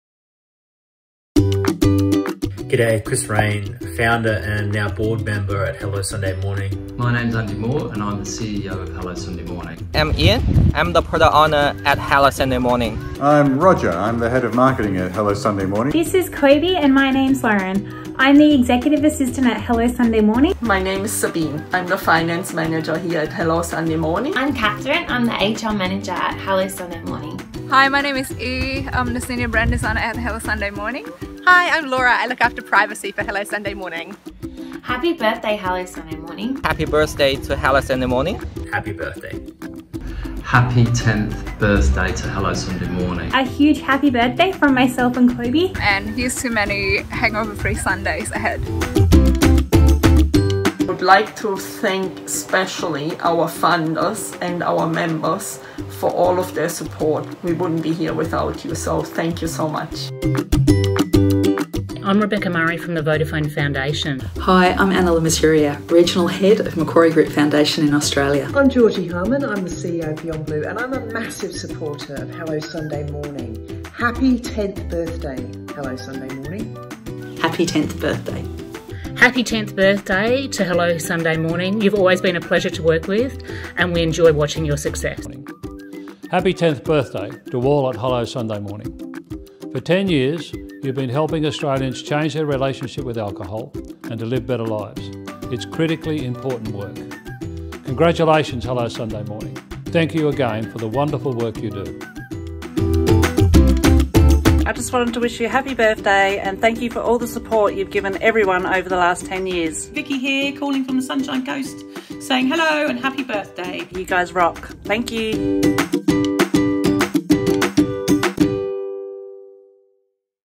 Happy 10th Birthday HSM!
It's our 10th anniversary of changing the world's relationship with alcohol. Watch our key staff and partners wish us a happy birthday!